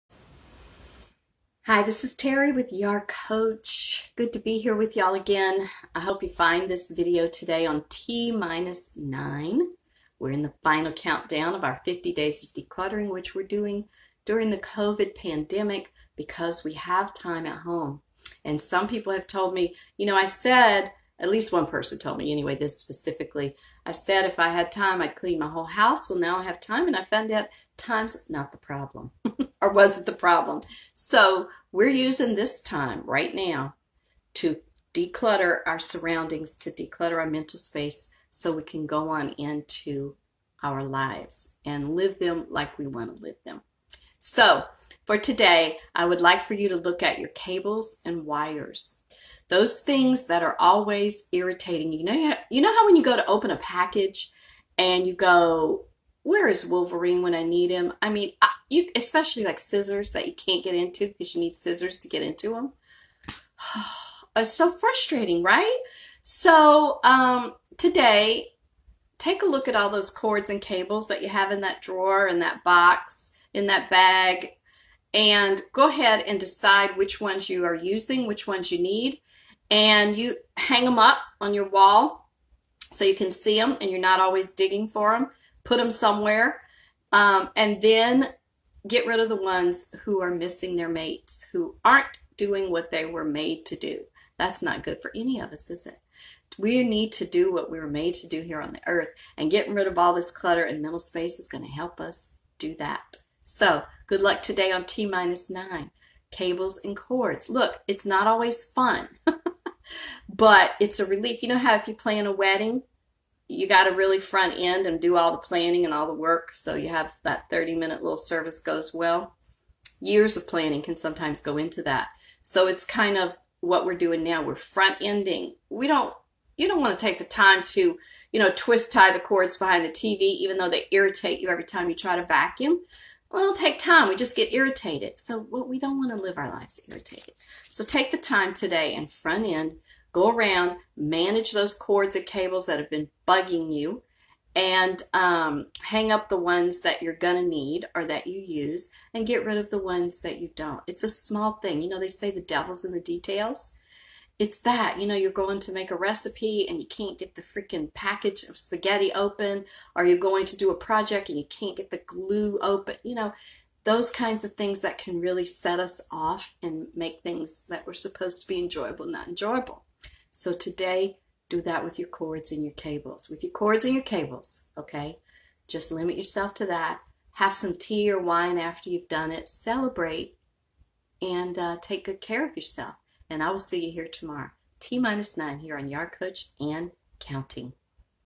Countdown: T-9! Decluttering Tips with Terri YAR Coach
T-9! Details! Details! Details!

Join me in 50 days of decluttering and reclaiming our personal and mental space. I’ll continue posting daily “assignments” on YAR Coach FB )and you’ll be able to find it on the YAR YouTube channel as well Tell your friends, involve the kiddos, feel good about doing what you can do in this time of limitation instead of feeling bad about all that you can’t do. And the best part is that we’ll all be doing it together. YAY!!!

Sending you so much love and hope for relief,
Terri